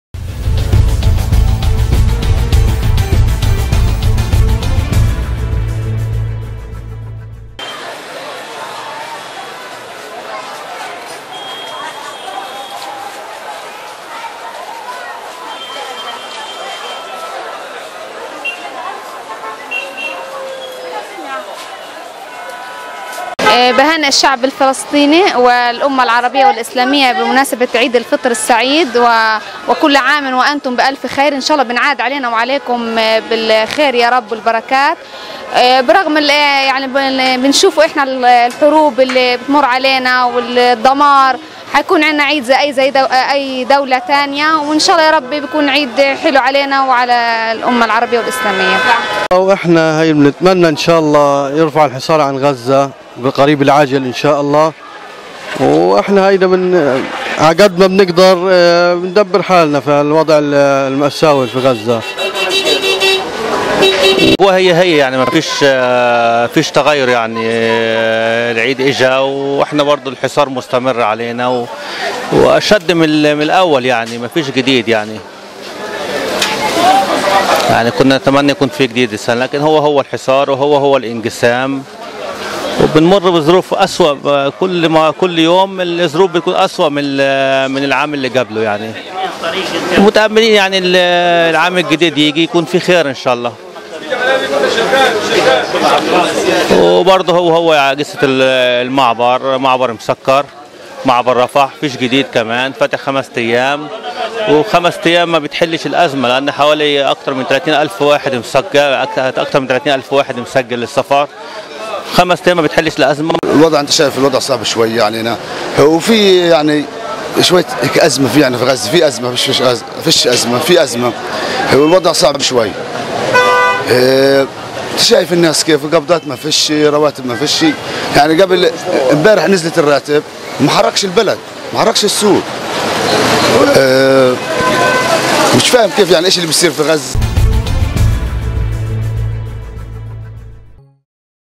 مصر العربية | الحصار يغيب أجواء العيد في غزة
مصر العربية | أجواء التحضير لعيد الفطر في غزة مغمسة بالحصار والفقر
فلسطين – مها عواودة
انعكست أجواء الحصار الإسرائيلي والفقر على استقبال عيد الفطر السعيد عند الغزيين , فالأسواق تشهد حالة ركود اقتصادي في ظل التحضيرات للعيد نتيجة الفقر والمعاناة التي يواجهها سكان قطاع غزة خاصة , وأن الحصار الذي طوي عامه العاشر قد انعكس بالسلب على مجمل الحياة الغزية " مصر العربية " ترصد في هذا التقرير تحضيرات الغزيين لعيد الفطر المبارك وكيف انعكست أجواء الحصار عليهم وقال المواطن الغزي محمد حماد ل" مصر العربية " :  " نحن نستقبل عيد الفطر السعيد في أجواء من الحصار والفقر المعابر كلها مغلقة , معبر رفح فتح لخمسة أيام وهي بالتأكيد أيام غير كافية لسفر أكثر من 30 ألف مواطن يودون السفر كلهم من الحالات الإنسانية ".